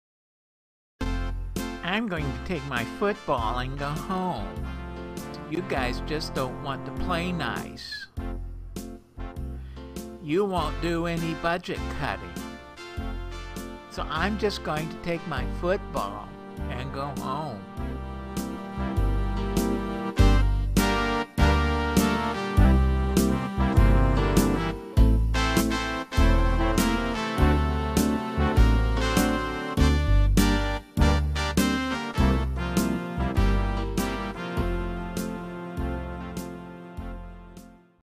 Eric Cantor takes his football and goes home
House Majority Leader shows his stuff as he takes his football and goes home.